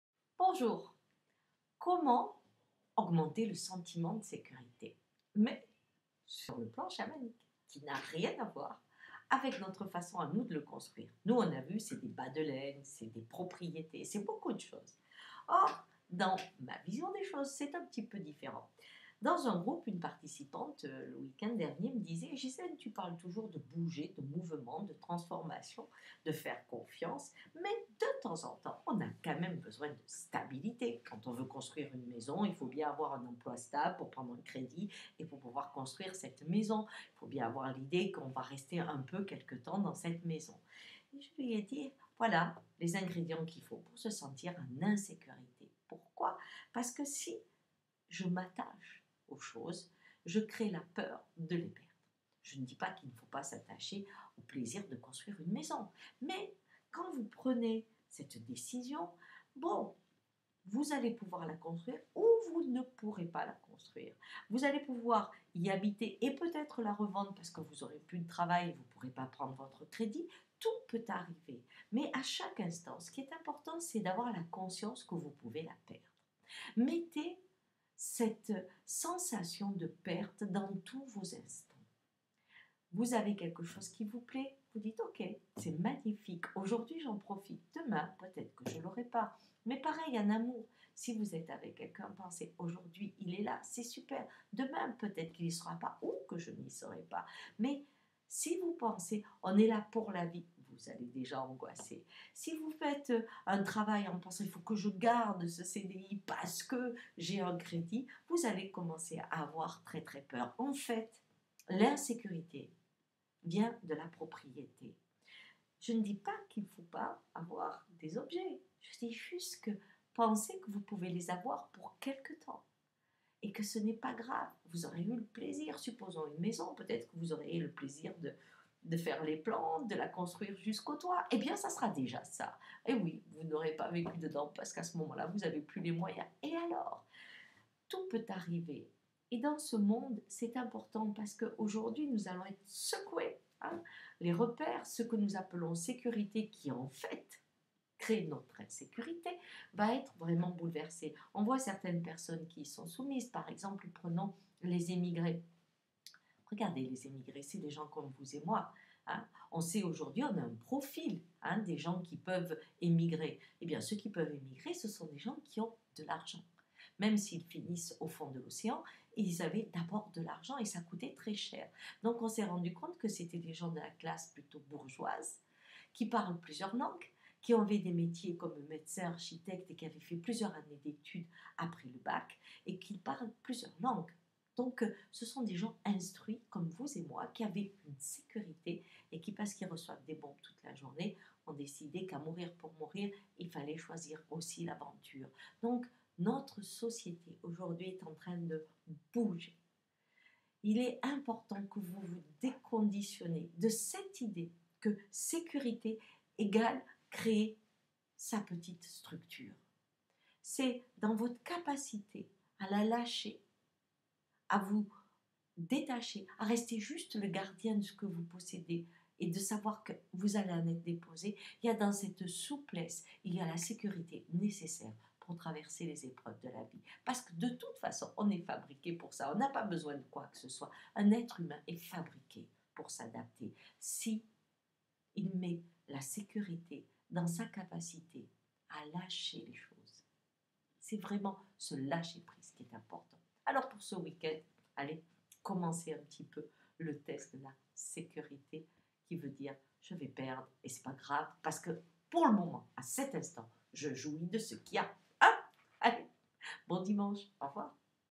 Comment améliorer son sentiment de sécurité ?
Parfois, on a besoin de se sentir stable pour se sentir en sécurité. Pourtant, dans la vision chamanique, c'est tout l'inverse qui produit la sécurité ! Découvrez pourquoi.